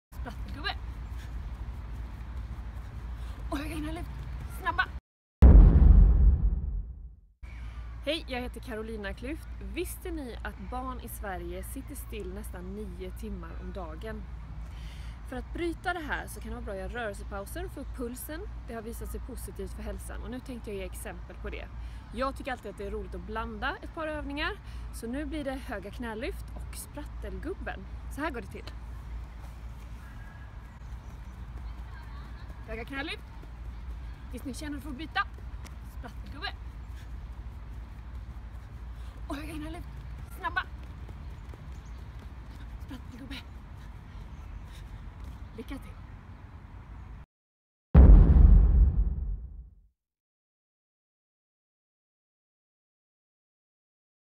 Upp och hoppa! Rörelsepaus med Carolina Klüft - Sprattelgubben
Vi sitter på tok för mycket stilla, hela 50 timmar i veckan. För mycket stillasittande leder till en rad olika hälsoproblem. Ett enkelt sätt att bryta de negativa effekterna är att minst en gång i halvtimmen ta en rörelsepaus. Här visar Carolina Klüft på en enkel övning som får upp pulsen på ett kick - sprattelgubben!